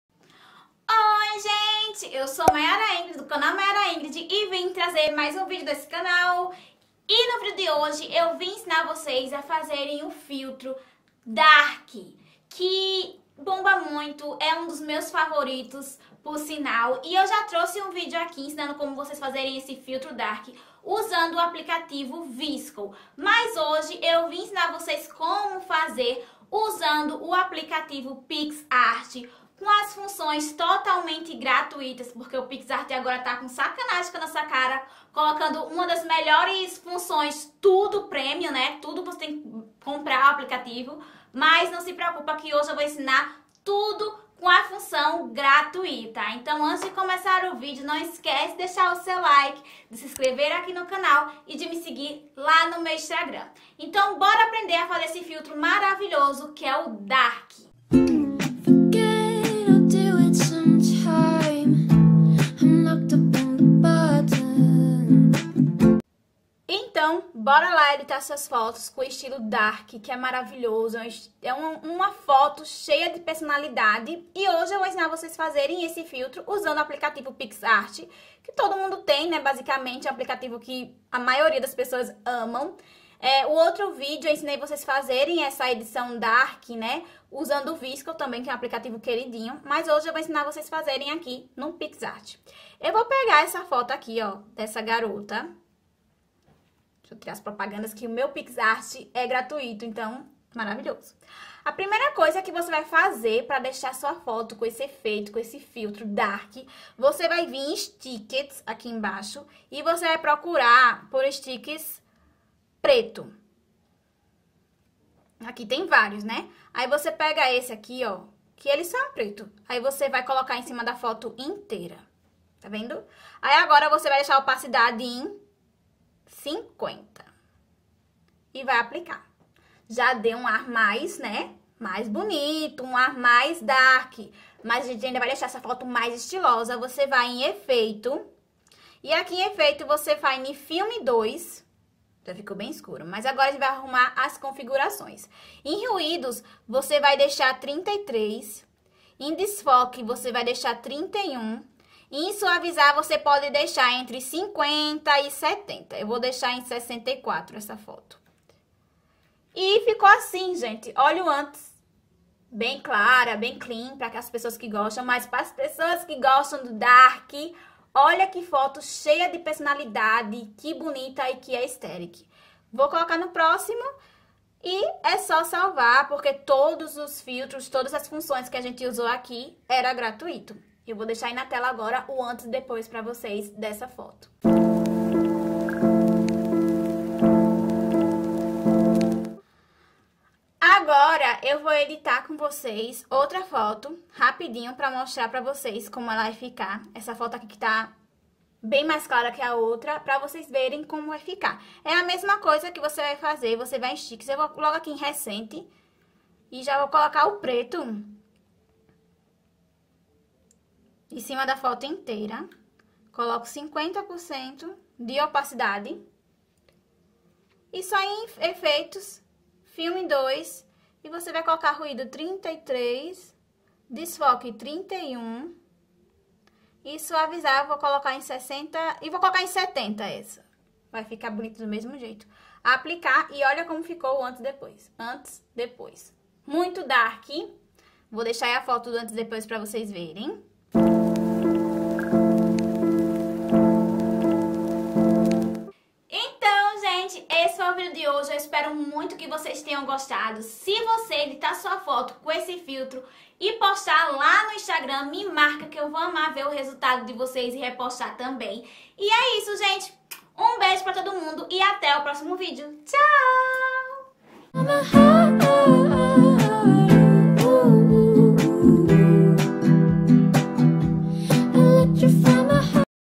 AESTHETIC DARK FILTER TUTORIAL PICSART EDIT ll como editar fotos estilo dark
Hey pessoal no video de hoje vim ensinar como editar suas fotos com o filtro DARK . esse estilo que estar super em alta no pinterest . se gostou desse video deixa seu like para mim trazer outro tipos de filtros .

Se você está lendo isso comente “Hello” é o nosso segredinho!😁

muitos muitos beijoos.